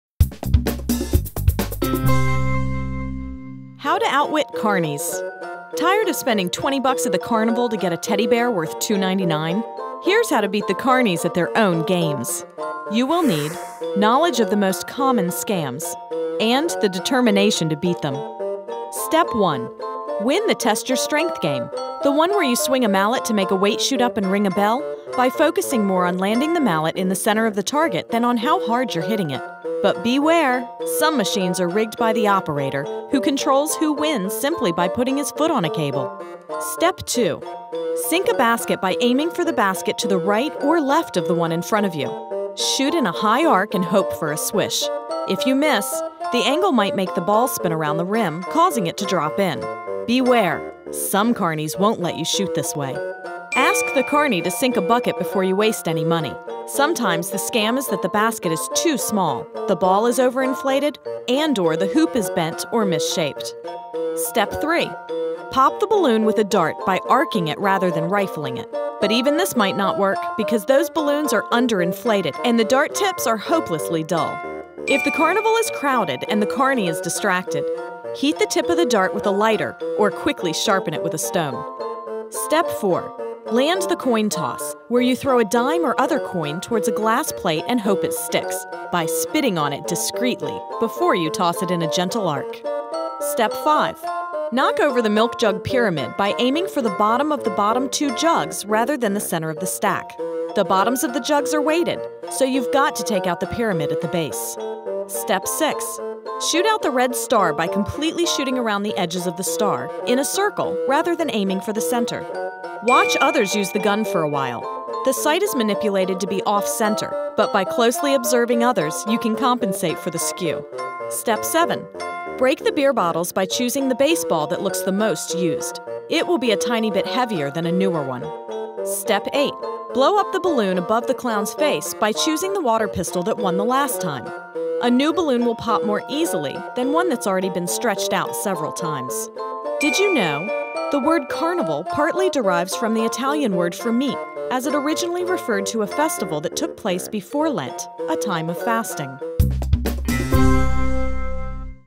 How to Outwit Carnies
Tired of spending $20 at the carnival to get a teddy bear worth $2.99? Here’s how to beat the carnies at their own games.

Step 1: Prove your strength
Win the 'test your strength' game—the one where you swing a mallet to make a weight shoot up and ring a bell—by focusing more on landing the mallet in the center of the target than on how hard you’re hitting it. But beware—some machines are rigged by the operator, who controls who wins simply by putting his foot on a cable.

Step 2: Sink a basket
Sink a basket by aiming for the basket to the right or left of the one in front of you. Shoot in a high arc and hope for a swish. If you miss, the angle might make the ball spin around the rim, causing it to drop in. Beware—some carneys won’t let you to shoot this way.

Tip
Ask the carnie to sink a bucket before you waste any money. Sometimes the scam is that the basket is too small, the ball is overinflated, and/or the hoop is bent or misshaped.

Step 3: Pop the balloon
Pop the balloon with a dart by arcing it rather than rifling it. But even this might not work, because those balloons are underinflated and the dart tips are hopelessly dull.

Tip
If the carnival is crowded and the carnie is distracted, heat the tip of the dart with a lighter, or quickly sharpen it with a stone.

Step 4: Land the coin toss
Land the coin toss—where you throw a dime or other coin toward a glass plate and hope it sticks—by spitting on it (discreetly!) before you toss it in a gentle arc.

Step 5: Knock over the milk jugs
Knock over the milk jug pyramid by aiming for the bottom of the bottom two jugs rather than the center of the stack. The bottoms of the jugs are weighted, so you’ve got to take out the pyramid at the base.

Step 6: Shoot out the star
Shoot out the red star by completely shooting around the edges of the star—in a circle—rather than aiming for the center.

Tip
Watch others use the gun for a while; the sight is manipulated to be off-center, but by closely observing others you can compensate for the skew.

Step 7: Break the beer bottles
Break the beer bottles by choosing the baseball that looks the most used; it will be a tiny bit heavier than a newer one.

Step 8: Blow up the balloon
Blow up the balloon above the clown’s face by choosing the water pistol that won the last time. A new balloon will pop more easily than one that’s already been stretched out several times.

The word 'carnival' partly derives from the Italian word for 'meat,' as it originally referred to a festival that took place before Lent, a time of fasting.